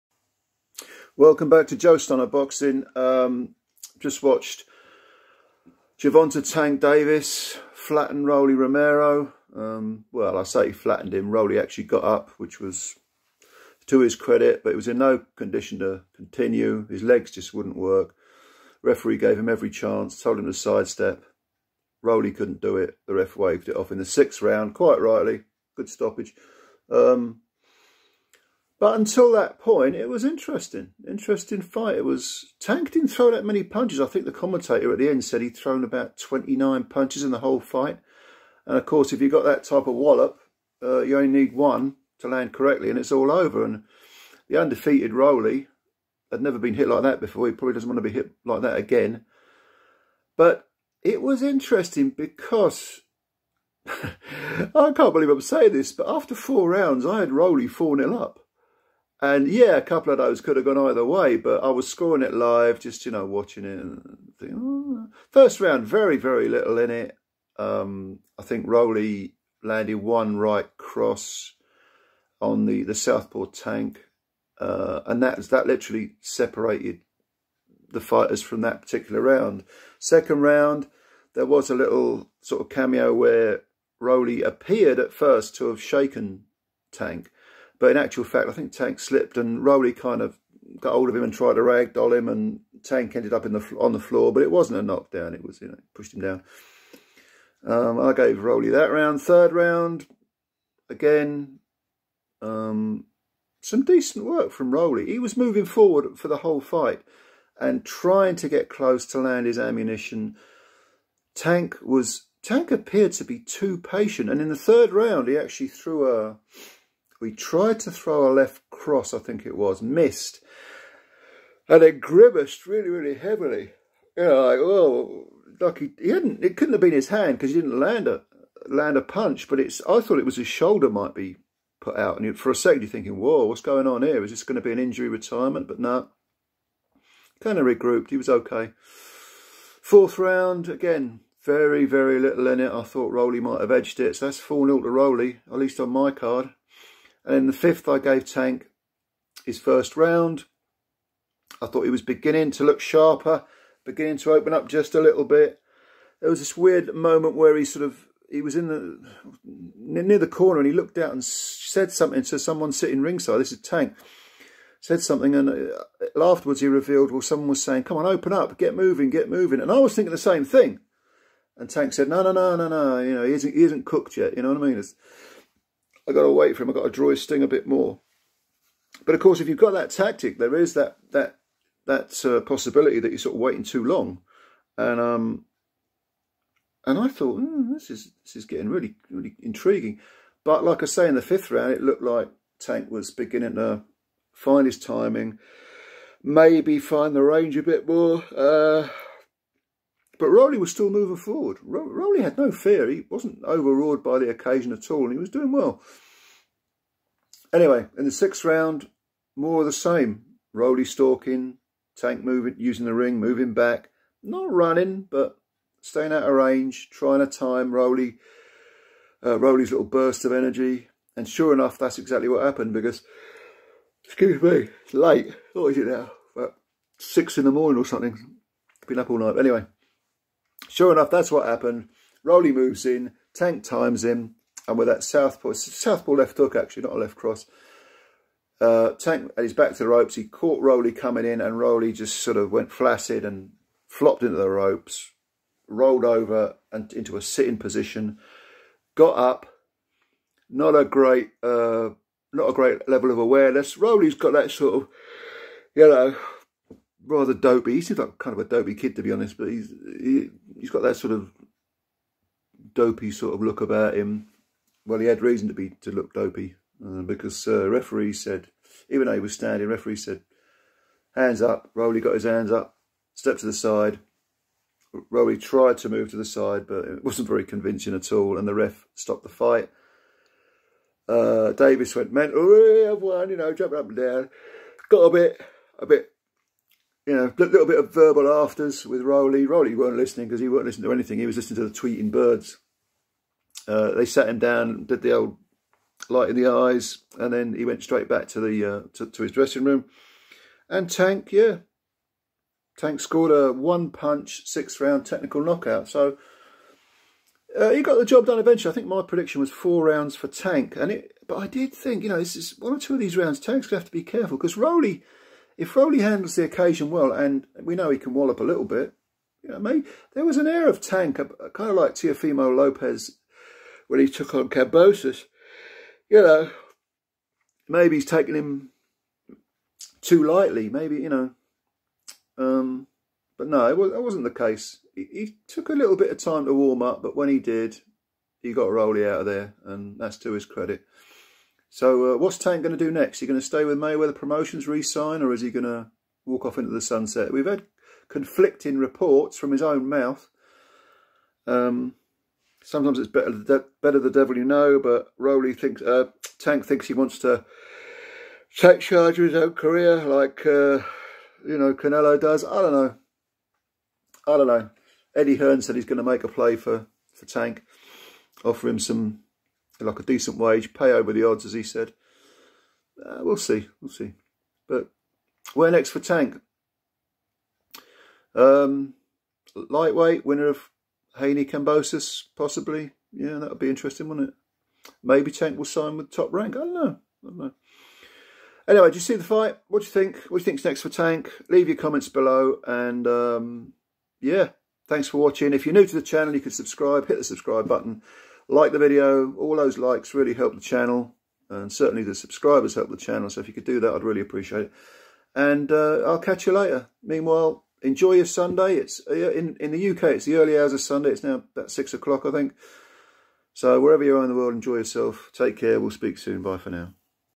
TANK steam-rollers ROLLY! GERVONTA DAVIS stops ROLANDO ROMERO in six rounds with a one-punch finish.
Patience was clearly a virtue for Gervonta "Tank" Davis as he handled some surprisingly controlled aggression from Rolando "Rolly" Romero to stop his unbeaten opponent with one punch in round six.  Rolly didn't embarrass himself but Tank's fire-power eventually ended a spirited challenge.